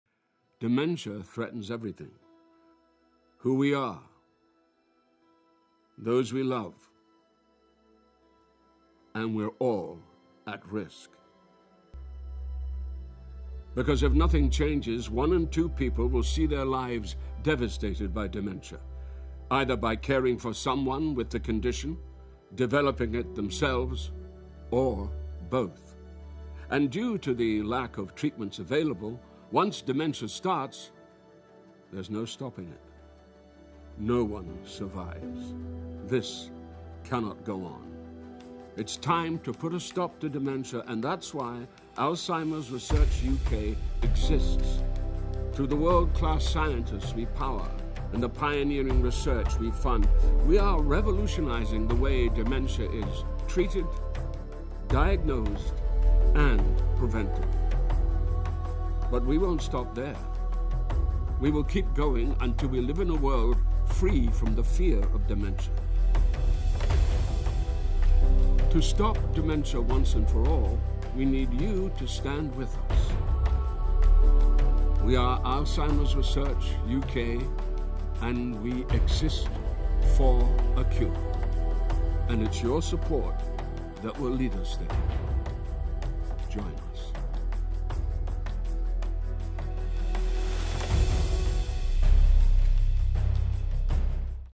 Alzheimer's Research UK - For A Cure
Almost one million people are living with dementia today. Tragically, not one of them will survive. Alzheimer’s Research UK exists to change that.

As the UK’s leading dementia research charity, we are working to revolutionise the way we treat, diagnose and prevent dementia. And then, we will find a way to cure it.

To do this, we’re investing in the best research, powering the most forward-thinking scientists and joining forces with world-class organisations.

With your support, we promise we will not stop until dementia can no longer destroy lives.